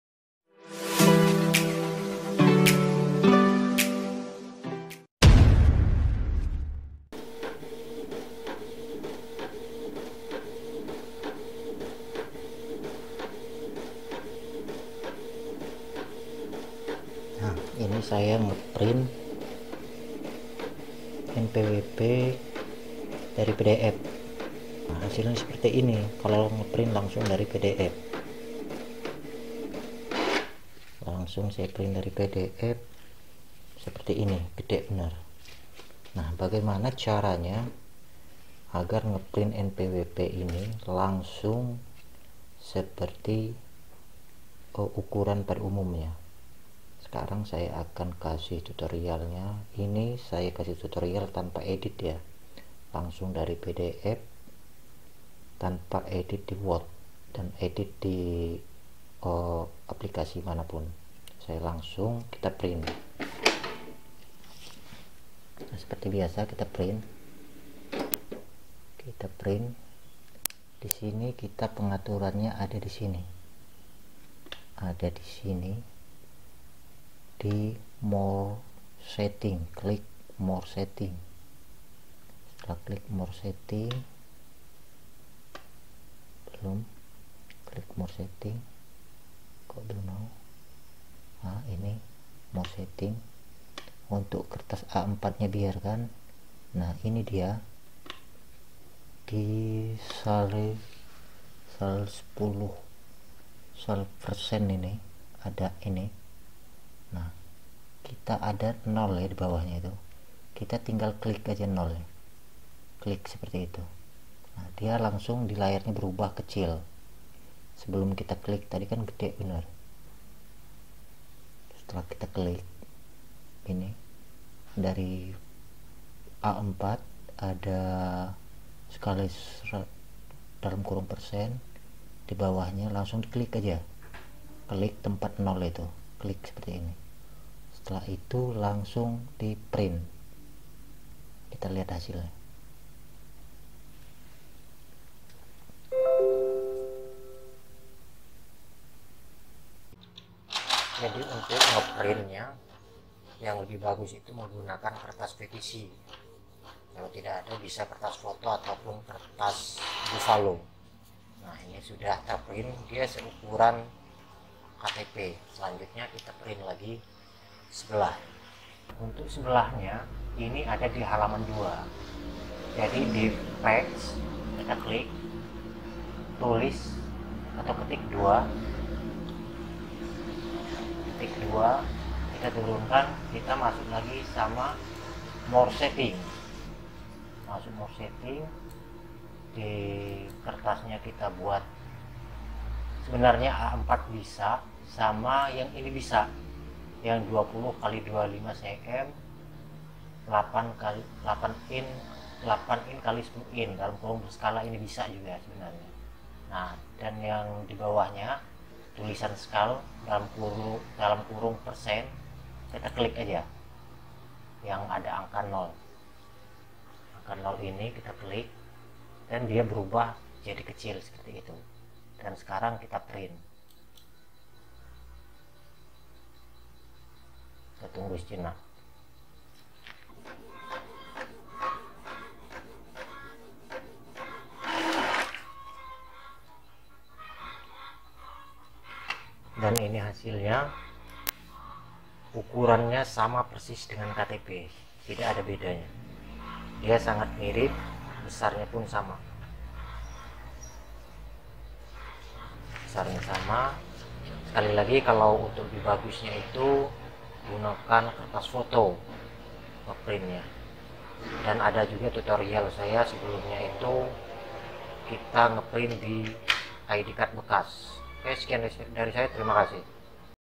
SEKALI klik!!! cetak NPWP dari file PDF sebesar KTP
mencetak NPWP sebesar KTP
ukuran KTP